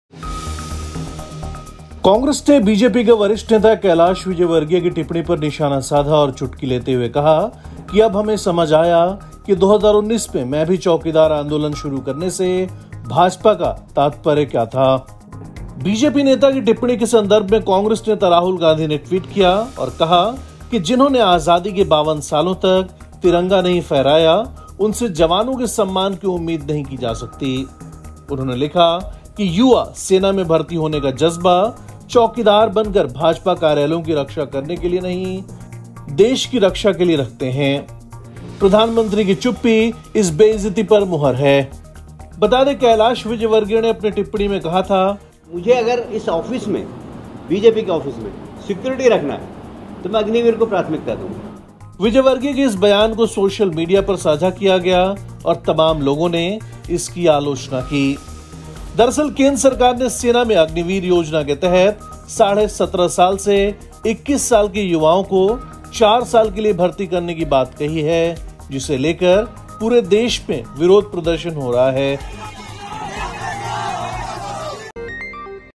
BJP पर Rahul Gandhi का तंज- अब समझ आया ‘मैं भी Chowkidar’ क्या था.
कांग्रेस ने BJP के वरिष्ठ नेता कैलाश विजयवर्गीय की टिप्पणी को लेकर निशाना साधा और चुटकी लेते हुए कहा कि अब ‘‘हमें समझ आया’’ 2019 में ‘मैं भी चौकीदार’ आंदोलन शुरू करने से भाजपा का तात्पर्य क्या था.